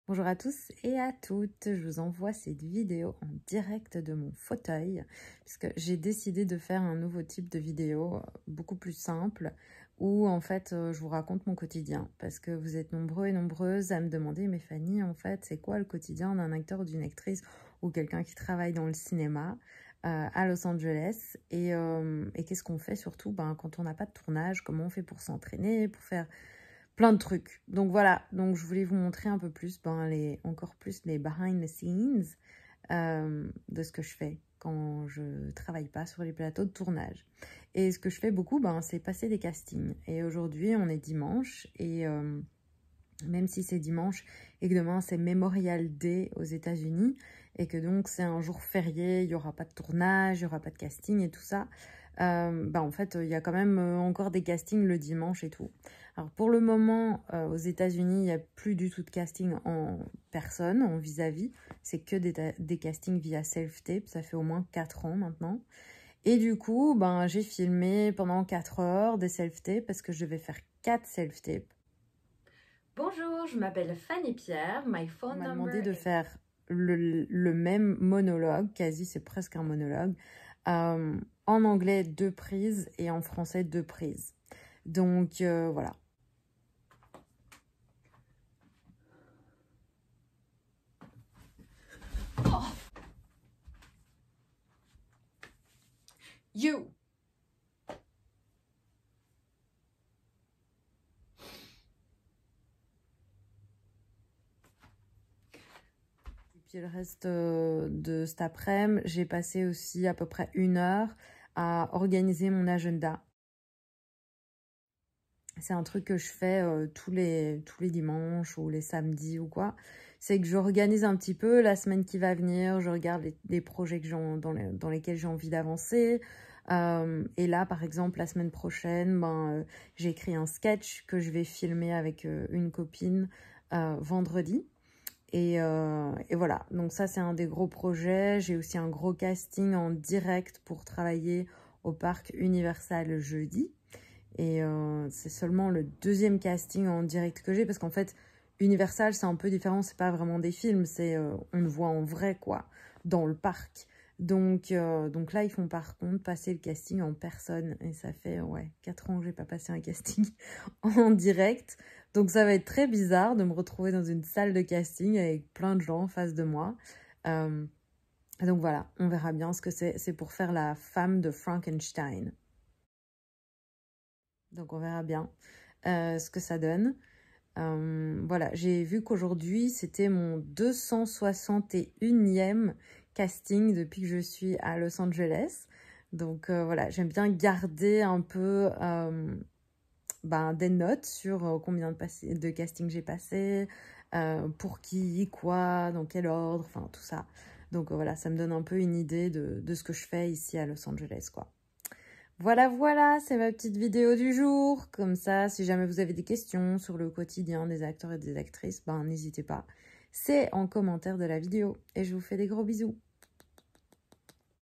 Vlog : mon quotidien d’actrice - 1
Je m’appelle Fanny Pierre, je suis une actrice Belgo-Americaine et je travaille dans le cinéma à Los Angeles aux Etats-Unis.